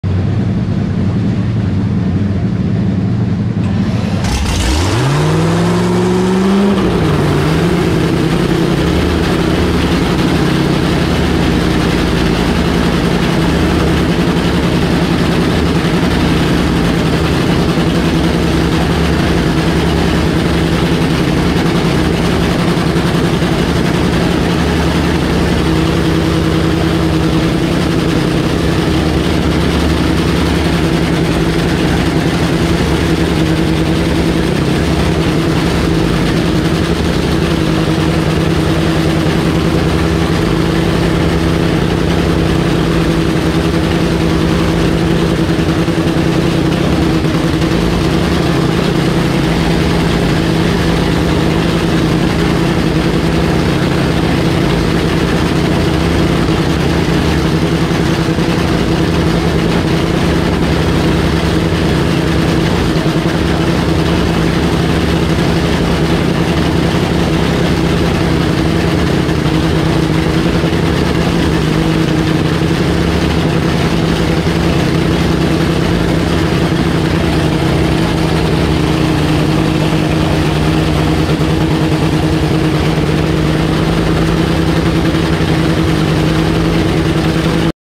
Matt Hagan Chest Cam Warmup
Now that's a warmup with a view! 👀 Get your CarolinaNats started with the sweet sweet sound of nitro! 🔥